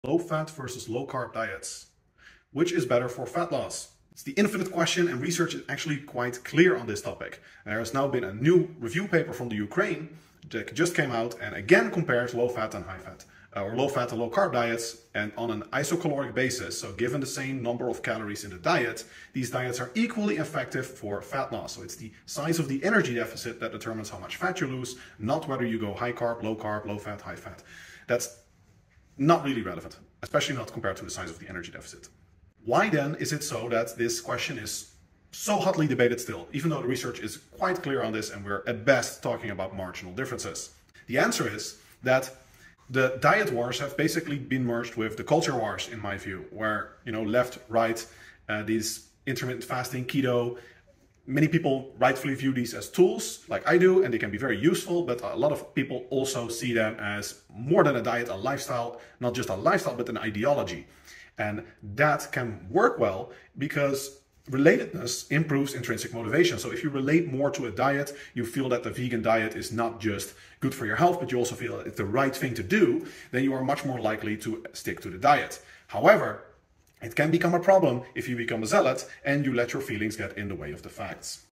Low-Carb vs Low-Fat Diets for Fat Loss in 90 seconds with Menno Henselmans
Ever wondered what the best diet for fat loss is? There is plenty of science-based research on the debate between low-carb vs low-fat diets. Learn more about it on this video with Menno Henselmans.